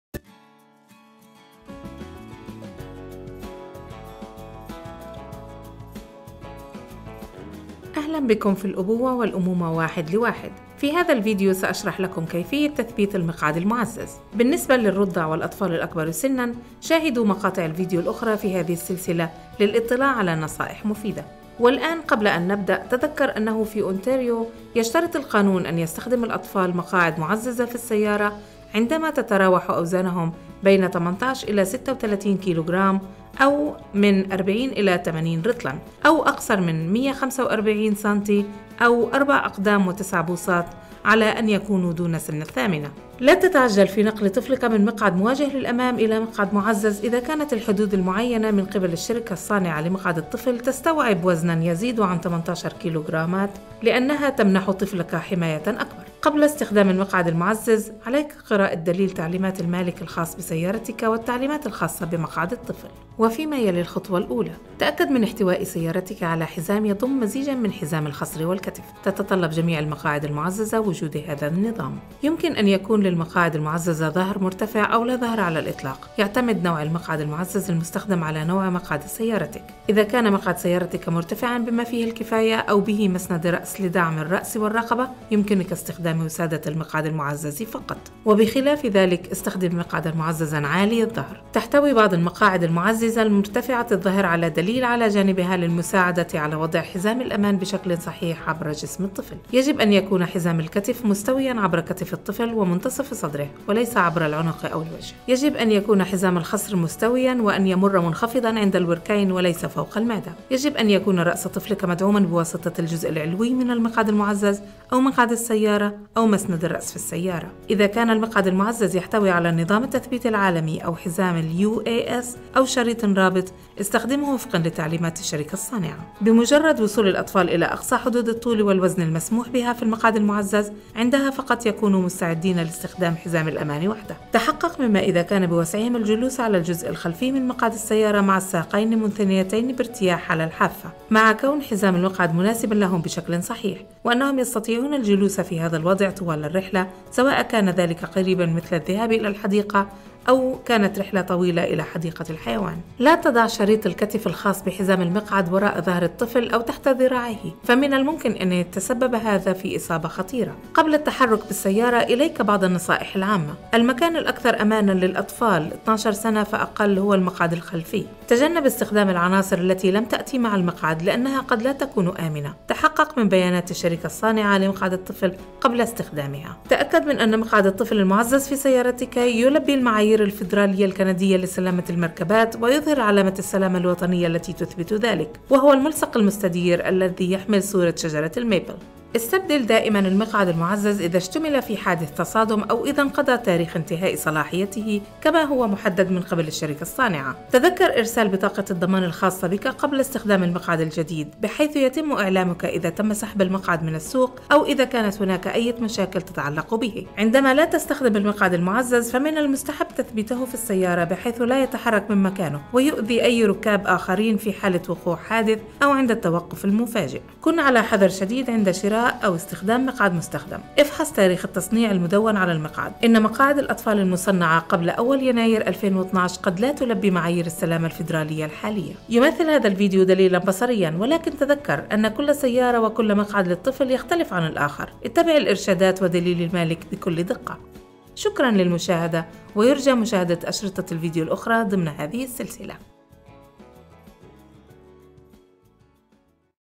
Guide - Child Booster Seat Installation - Arabic language
Child Booster Seat Installation - this video guides you through in Arabic language. This road safety project was undertaken by 'Safety Drives Us' committee with the assistance of Ontario Ministry of Transportation and other road safety partners.